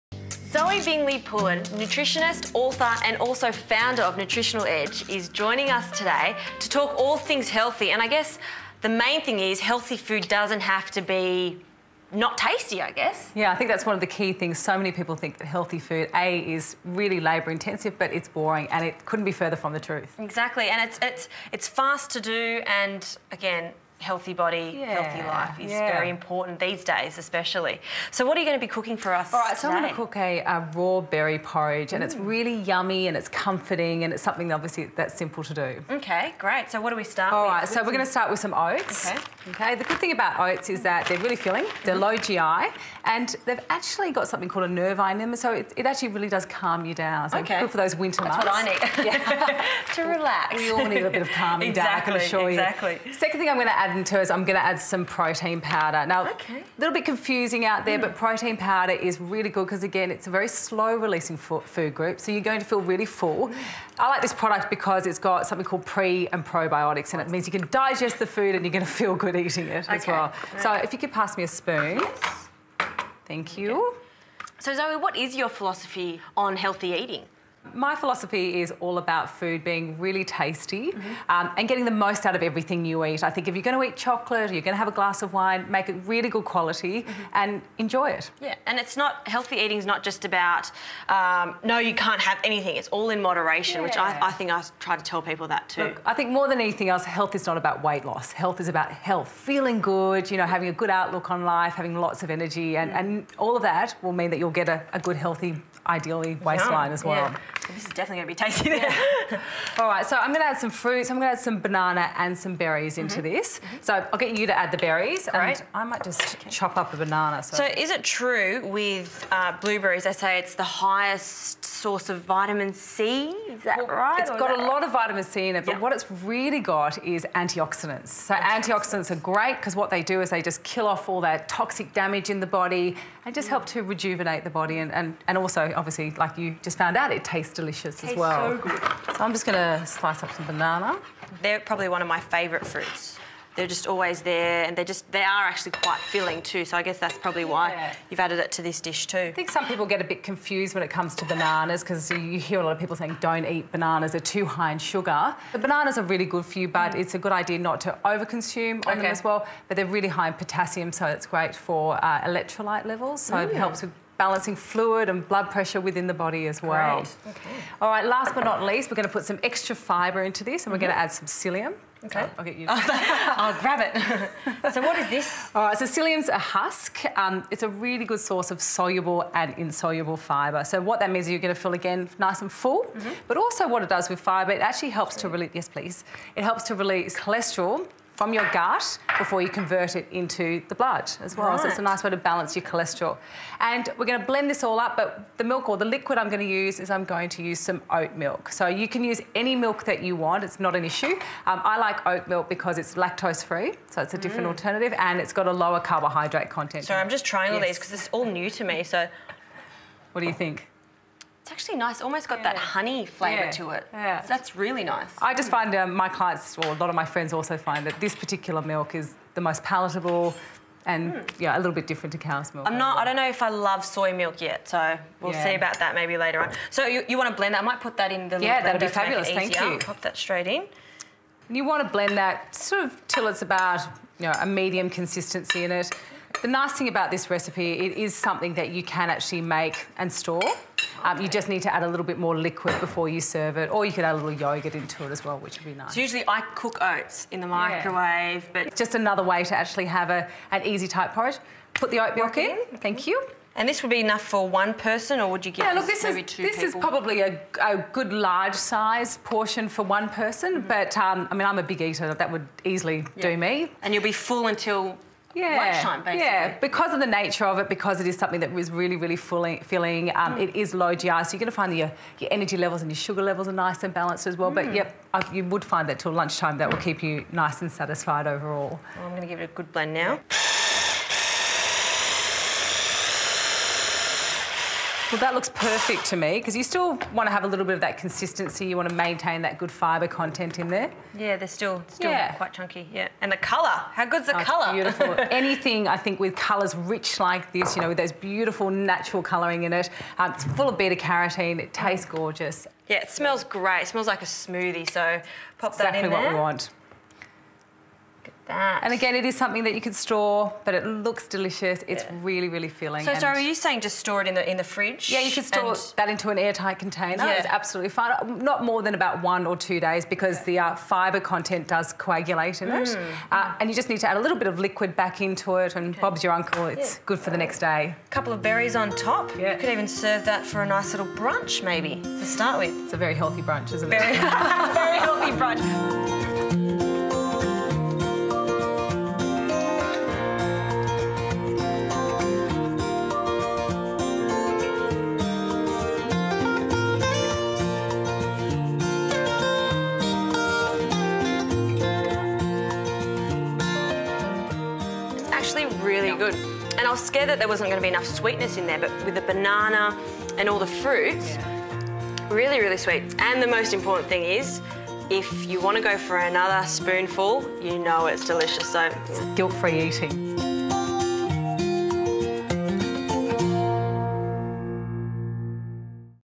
EG - Raw Berry Porridge April, 12.m4v
Do you want a quick and healthy breakfast, try my raw berry porridge.